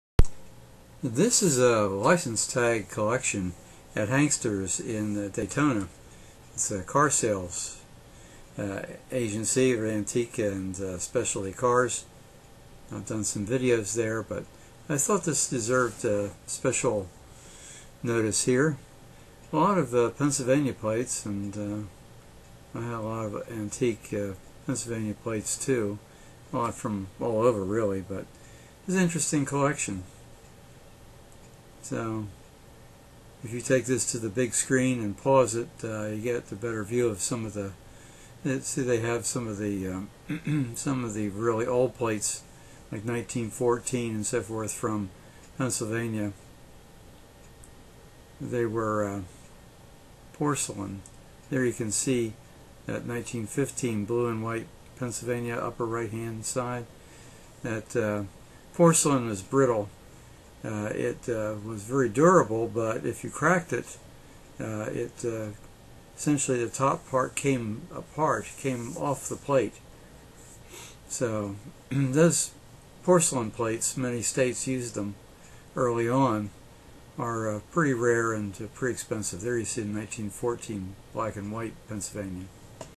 Antique Car Plates Hanksters Daytona0412184441
For those of you who collect license plates, you might be interested in this collection at Hankster's shop.  Many are modern, but some go back to the teens.  Those who are interested in collection might check out ALPCA...the website dedicated to collecting plates from all over the world.
Show less